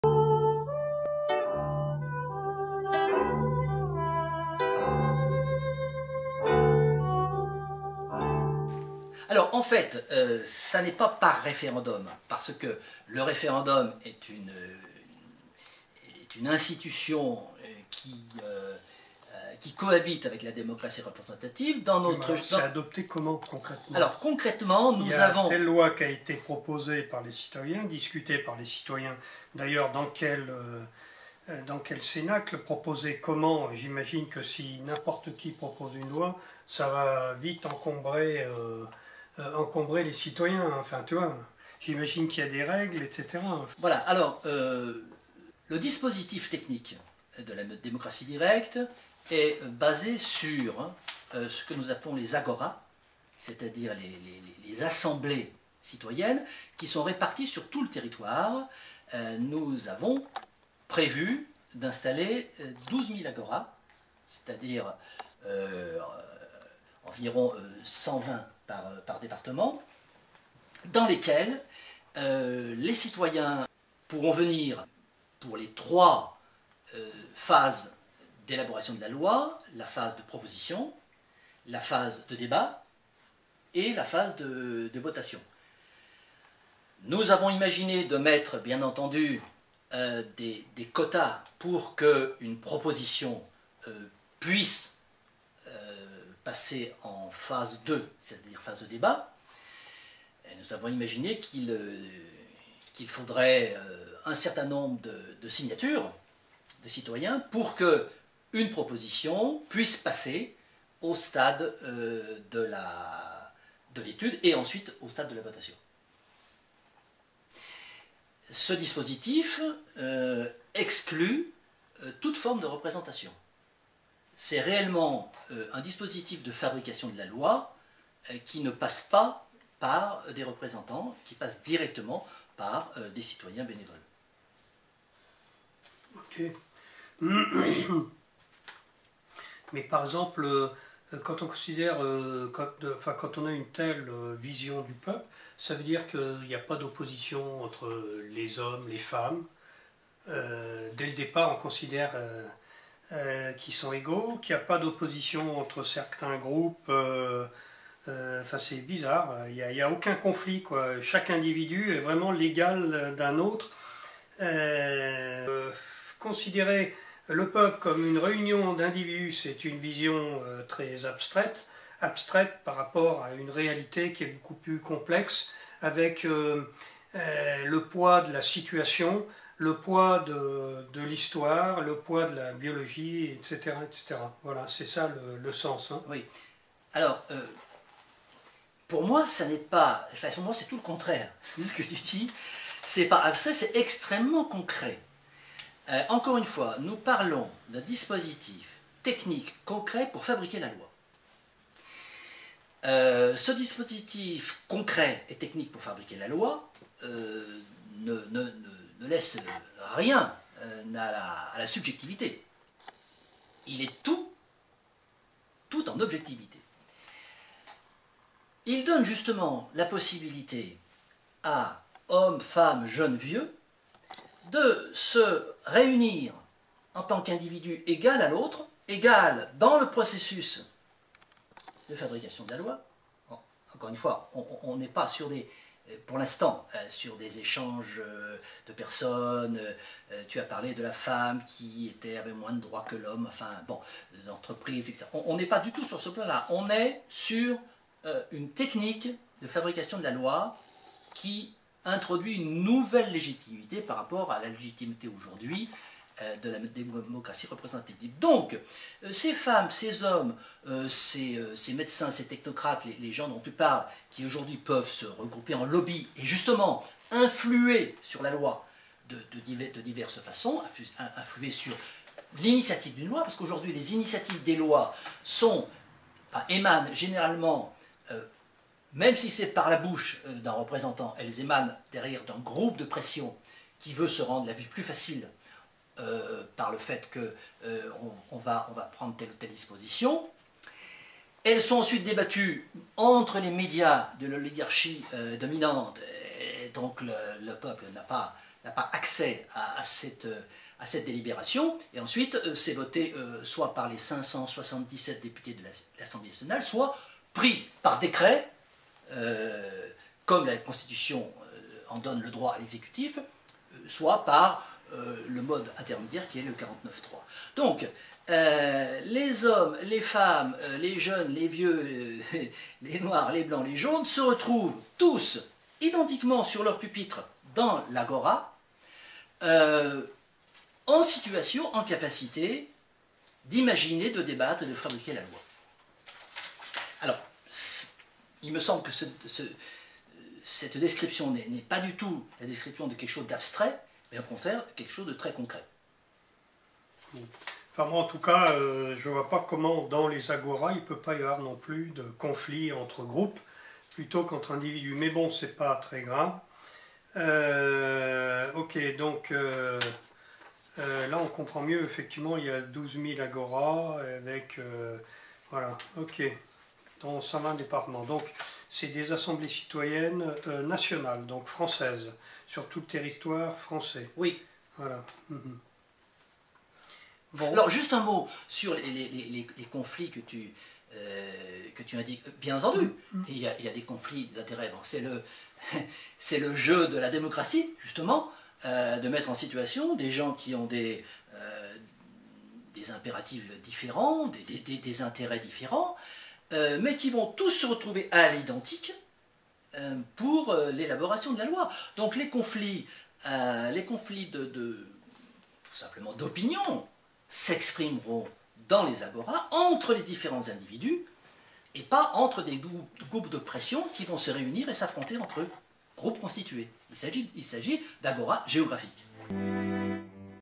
Christian Laurut : A propos du référendum
Débat entre un objecteur de croissance et Christian Laurut autour de son livre "Vers la démocratie directe"